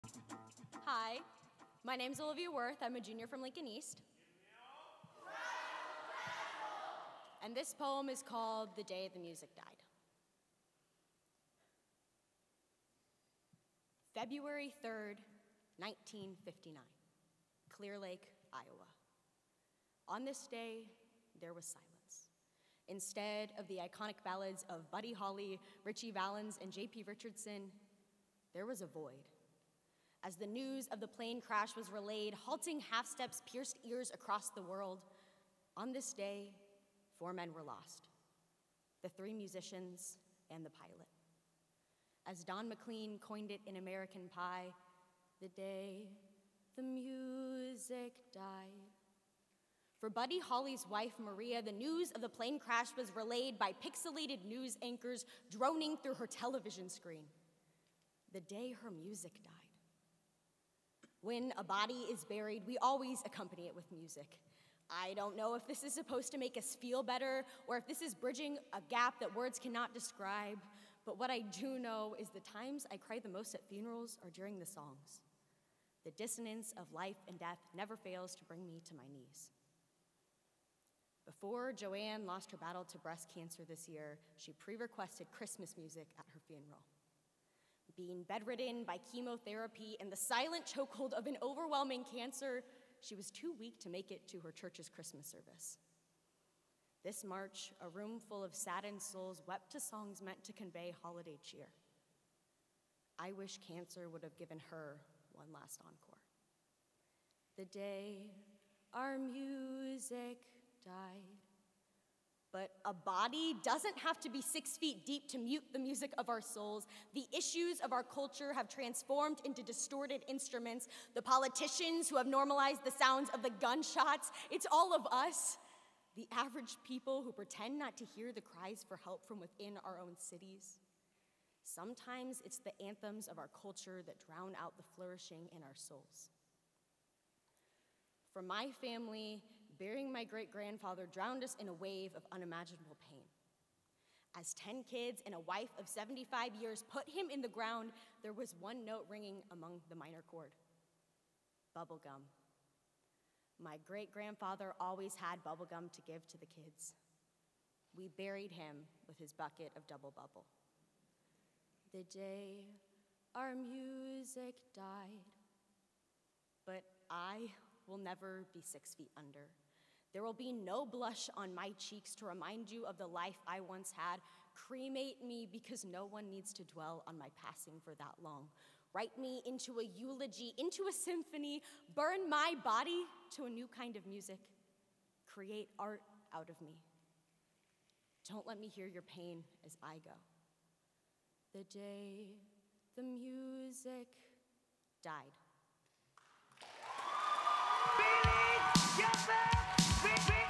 Louder Than a Bomb Great Plains, 2018 Finals
Louder Than a Bomb Great Plains, 2018 Finals April 24th at the Holland Center for Performing Arts.

Featuring teams from: Lincoln High School, Omaha Northwest, Westside and Lincoln East.